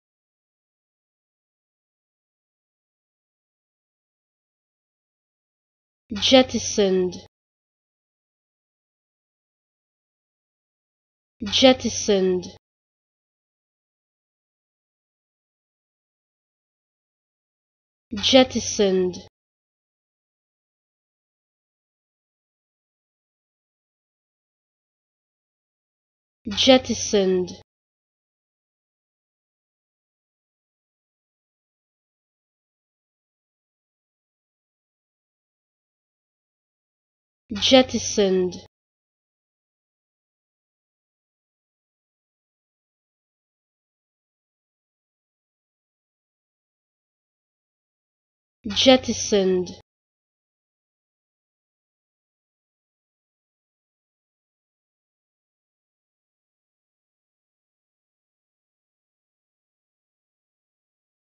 How To Pronounce Jettisoned
Learn how to say Jettisoned with EmmaSaying free pronunciation tutorials.